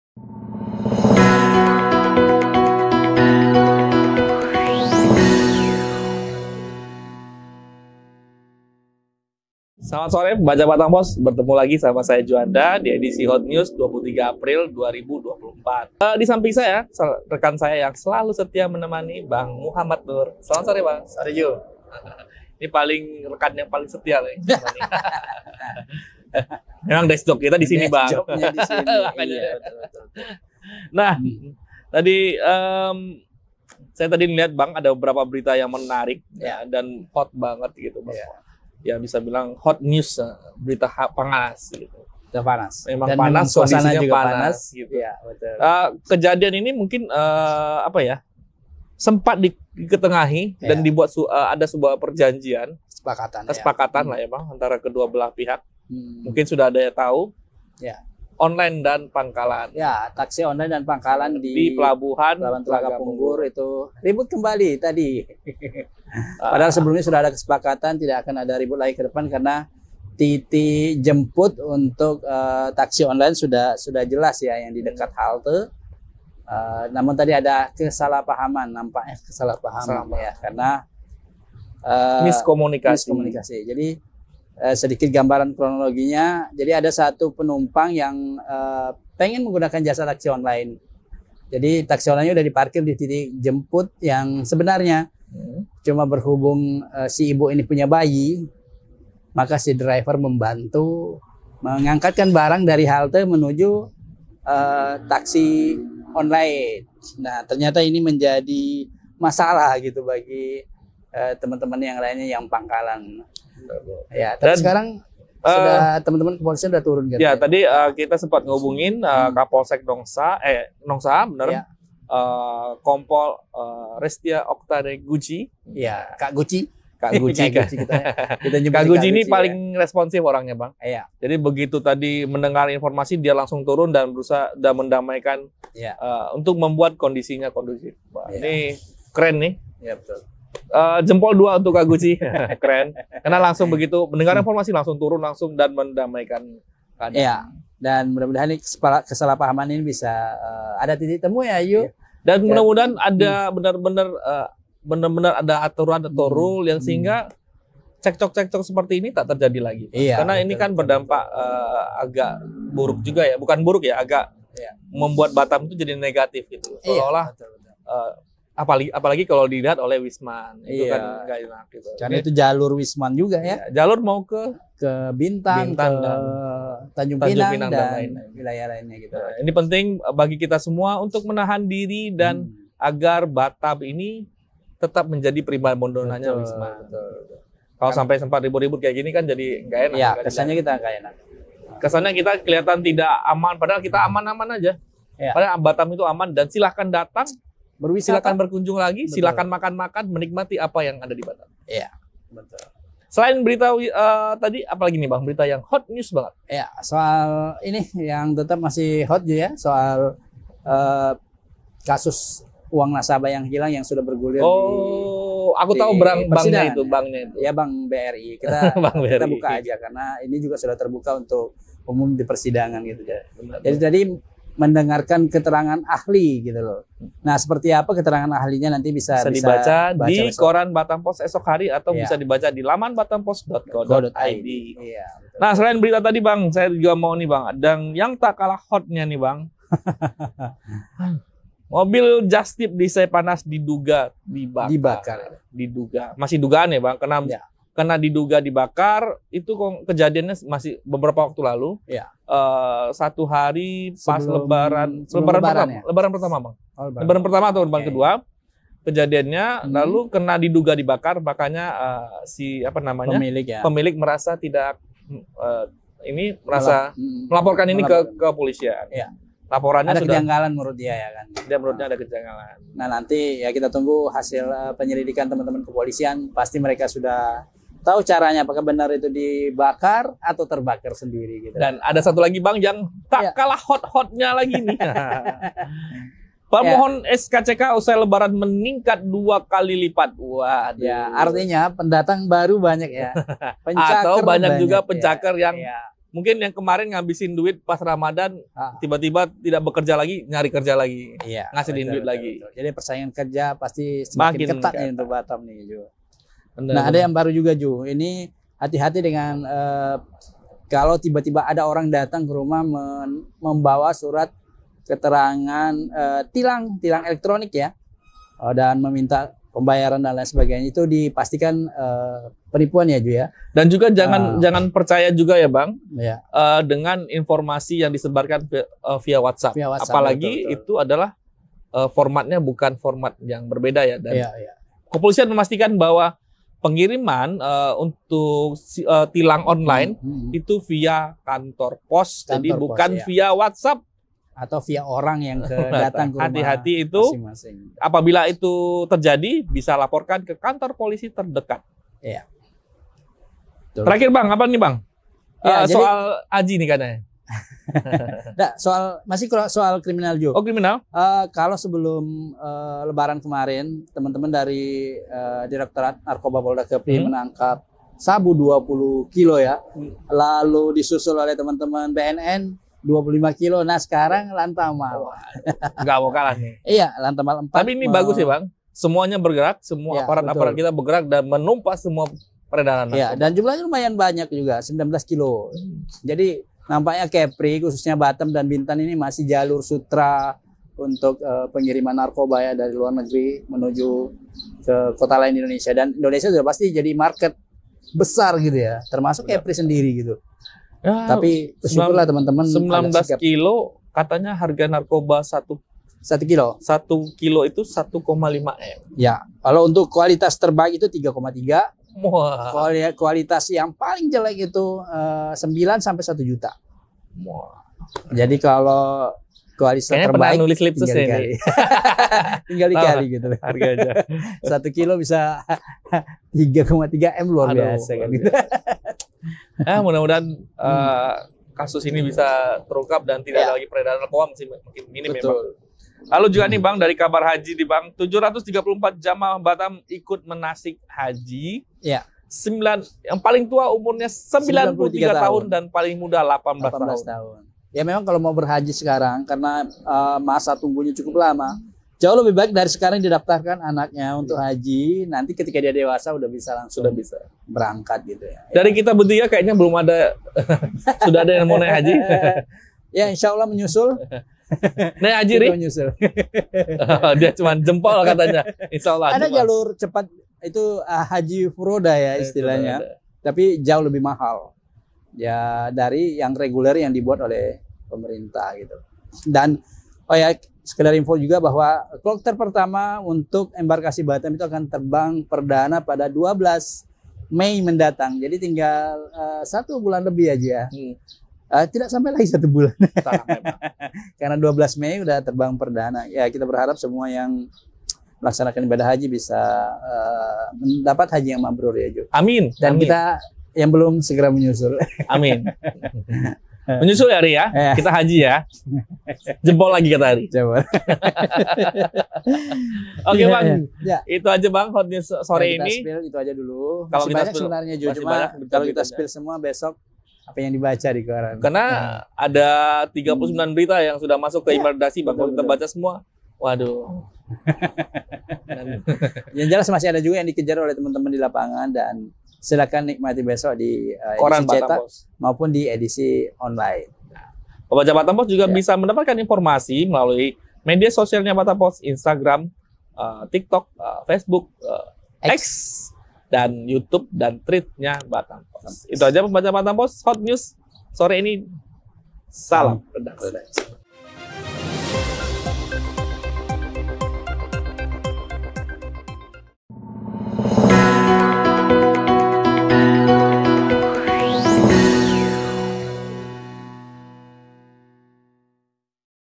Driver Online & Pangkalan Ribut di Pelabuhan Telaga Punggur Hotnews Batam Pos Edisi 23 April 2024
Rancangan berita terbit di Harian Batam Pos Edisi 23 April 2024.
- Pembelian Pertalite dengan Fuel Card akan Dibatasi.
- Sidang BRI: Keterangan Ahli Menjadi Sorotan.
- Waspada Penipuan, Bukti ETLE Harus Melalui Kantor Pos, Bukan WhatsApp.
- Mobil Jasa Angkutan Diduga Dibakar di Seipanas, Polisi Menyelidiki Kasus.
- Tangkapan Lantamal IV: 19 Kg Sabu Disita.
- Pemohon SKCK Meningkat 2 Kali Lipat Pasca Lebaran.
- Dinkes Mengingatkan Bahaya DBD di Musim Pancaroba.
- 734 Jemaah Batam Ikuti Manasik Haji, Termasuk Orang Tertua Berusia 93 Tahun.
- Perselisihan Antara Driver Online dan Pangkalan di Pelabuhan Telagapunggur.

Official Social Media of Batam Pos